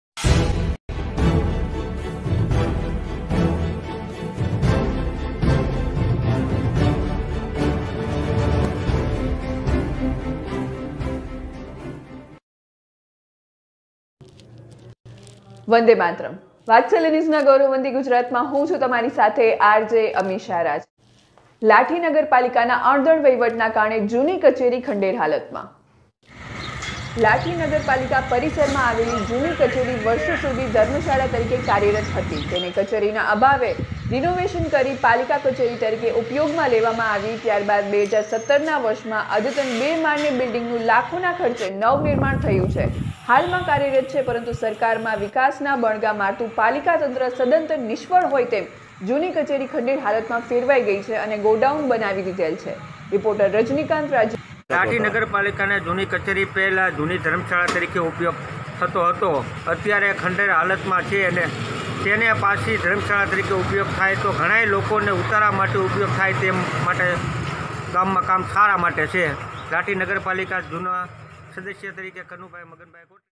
લાઠી નગરપાલિકાના અણધણ વહીવટના કારણે જુની કચેરી ખંઢેર હાલતમાં // Lathi Nagarpalika
લાઠી નગરપાલિકાના અણધણ વહીવટના કારણે જુની કચેરી ખંઢેર હાલતમાં: પહેલા ધમૅશાળા હતી.

લાઠી નગરપાલિકા પરિસરમાં આવેલી જુની કચેરી વષોૅ સુધી ધમૅશાળા તરીકે કાર્યરત હતી.તેને કચેરીના અભાવે રીનોવેશન કરી પાલીકા કચેરી તરીકે ઉપયોગ મા લેવામાં આવી ત્યારબાદ ૨૦૧૭ ના વષૅમાં અદ્યતન બે માળની બિલ્ડિંગનુ લાખોના ખર્ચે નવનિર્માણ થયુ છે હાલમાં કાયૅરત છે.પરંતુ સરકારમાં વિકાસના બણગા મારતું પાલીકા તંત્ર સદંતર નિષ્ફળ હોય તેમ જુની કચેરી ખંઢેર હાલતમાં ફેરવાઈ ગઈ છે.અને ગોડાઉન બનાવી દિધેલ છે.લાઠીમા આવતા યાત્રાળુઓ અને ભિક્ષુકો માટે વળીયા કુટંબના દાતાએ આ બિલ્ડીંગ ધમૅશાળામા ઉપયોગમાં લેવાય તે હેતુથી બનાવી આપેલ હતુ જે એકમાત્ર આશરાનુ સ્થાન હતું.જેને ખંઢેર હાલતમાં જોઈ લોકોમાં પણ રોષ જોવા મળે છે.બીજી બાજુ જુની કચેરીના પરિસરને જાણે કે  પાકીૅગ બનાવી દીધેલ હોય તેમ વાહનો અને ટુ-વ્હીલર ના થપ્પા લાગી જાય છે.છતાં પાલીકા તંત્ર ઘોર નિંદ્રામાં છે.કચરાપેટીના બંધ પડેલ જુના વાહનોના થડકલા જામ્યા છે છતાં ભાજપનો વિકાસ ગાંડો થયો હોય તેવું લાગે છે.વષોૅથી પાલીકામા રેગ્યુલર ચીફ ઓફિસર નથી પ્રોફેશનલ ઉપર નિવૃત્ત કમૅચારીની નિમણુક કરવામાં આવે છે તેને બન્ને બાજુથી ઘી કેળા પેન્શન અને સેલરી મળી ૪૦ થી ૫૦ હજાર રૂપિયા ની બેઠી આવક પછી ગામની ચિંતા શું ?આમા બેરોજગારી દુર થાય ક્યાંથી.

રજનીકાંત રાજ્યગુરૂ લાઠી.રીપોટૅર